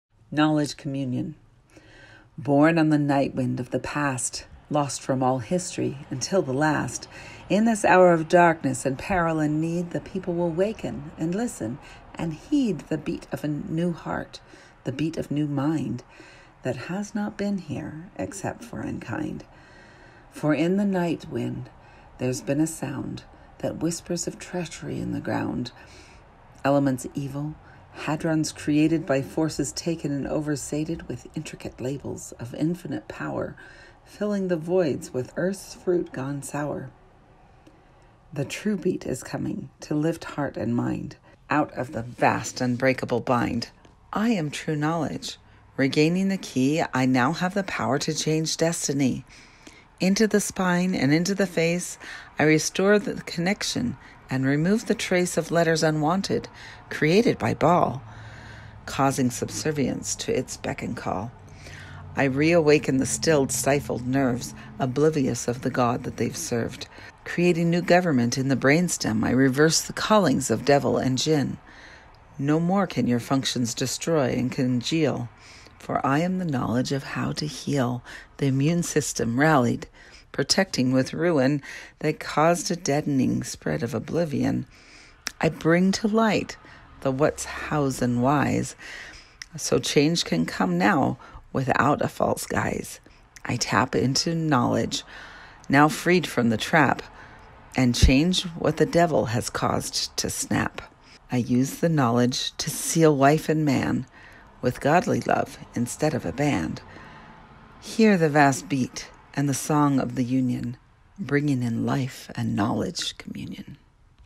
Knowledge Communion
Bourne on the night wind of the past
Lost from all history until the last –
In this hour of darkness and peril and need
The people will waken and listen and heed
The beat of new heart, the beat of new mind
That has not been here, except for in kind.
For in the night wind there's been a sound
That whispers of treachery in the ground.
Elements evil, hadrons created
By forces taken and oversated
With intricate labels of infinite power,
Filling the voids with earth’s fruit gone sour.
The true beat is coming to lift heart and mind
Out of the vast unbreakable bind.
I am pure knowledge. Regaining the key,
I now have the power to change destiny.
Into the spine and into the face,
I restore the connection and remove the trace
Of letters unwanted, created by Ba’al,
Causing subservience to its beck and call.
I reawaken the stilled, stifled nerves
Oblivious of the god that they’ve served.
Creating new government in the brainstem,
I reverse the callings of devil and jinn.
No more can your functions destroy and congeal,
For I am the knowledge of how to heal.
The immune system rallied, protecting with Ruin,
That caused a deadening, spread of oblivion,
I bring to light the whats, hows and whys
So change can come now without a false guise.
I tap into the knowledge now freed from the trap
And change what the devil has caused to snap.
I use the knowledge to seal wife and man.
With godly love, instead of a band.
Hear the vast beat and the song of the union
Bringing in Life and Knowledge communion.